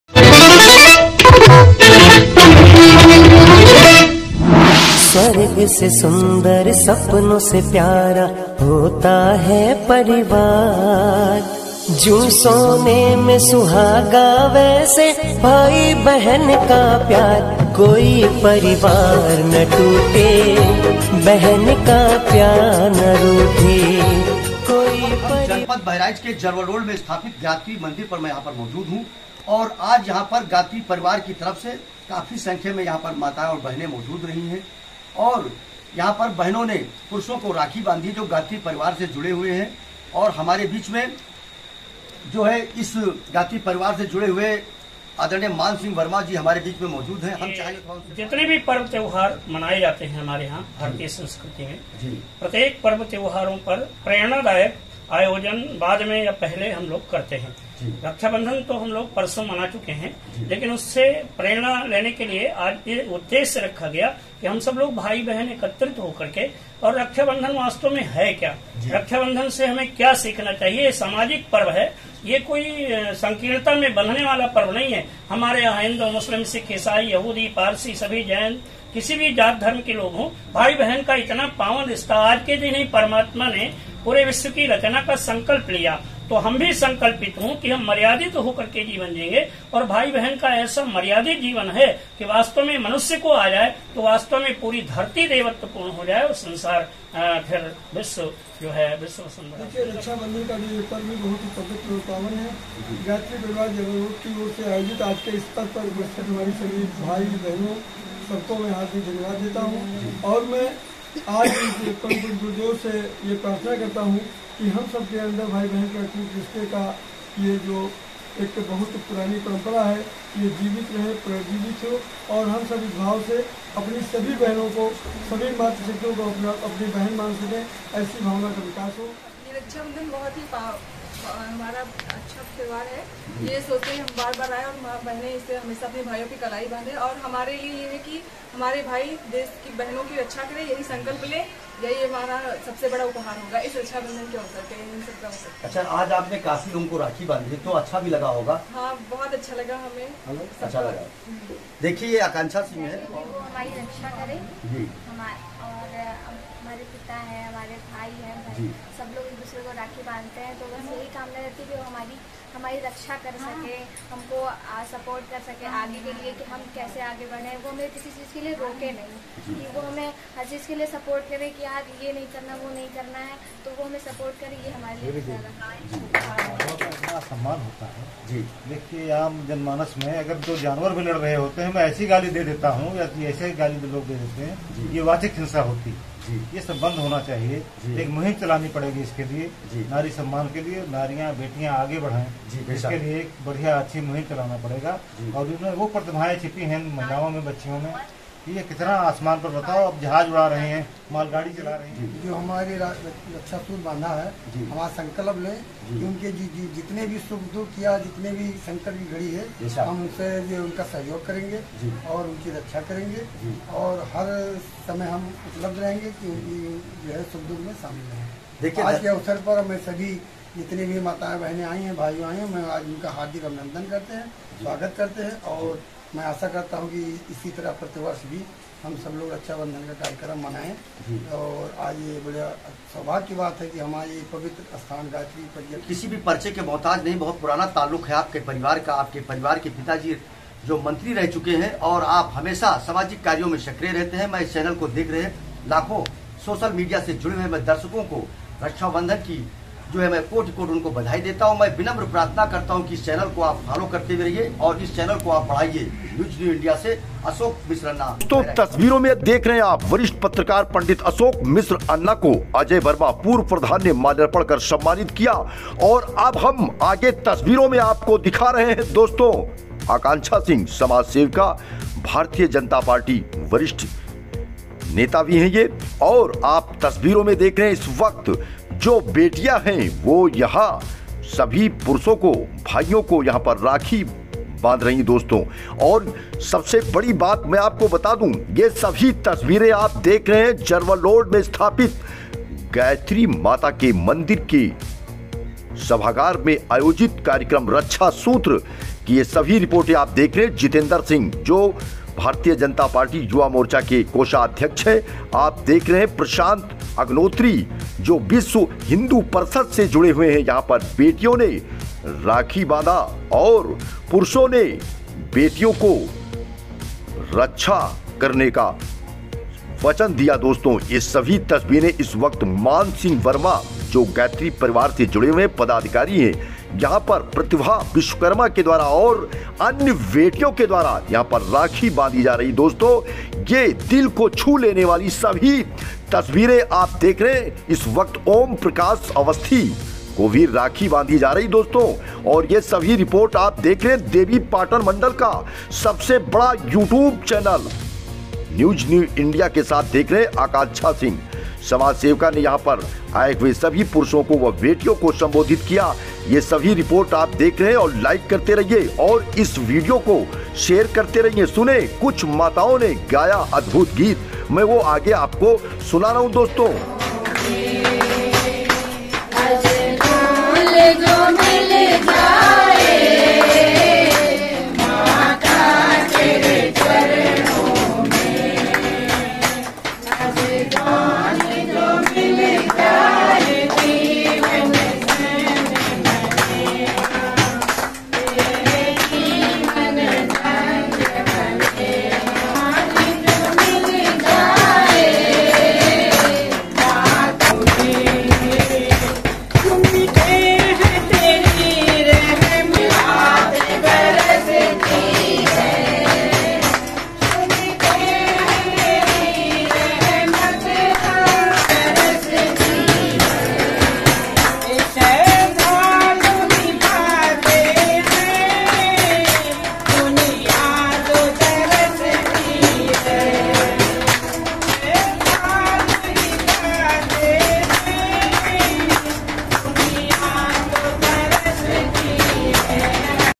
जरवलरोड बहराइच- गायत्री मंदिर पर आयोजित रक्षा सूत्र कार्यक्रम/
मानसिंह वर्मा गायत्री परिवार वरिष्ठ पदाधिकारी, देखें संक्षिप्त वीडियो रिपोर्ट वरिष्ठ पत्रकार पंडित अशोक मिश्र "अन्ना भैय्या" के साथ मोबाइल नंबर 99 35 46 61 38/ जनहित में जारी √√